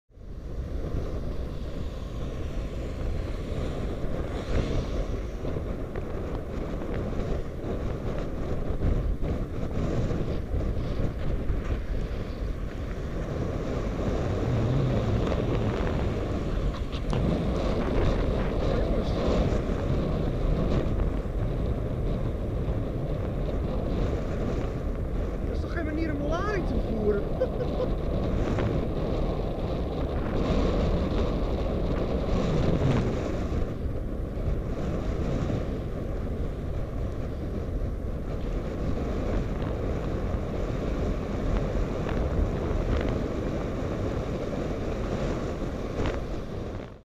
What a drag - Helmetcam
Early in the morning I heard some strange sounds... scraping sounds. What would make these kinds of noises? A driver with a strange idea of securing a load.

Camera: Contour+
Bike: Sinner Mango+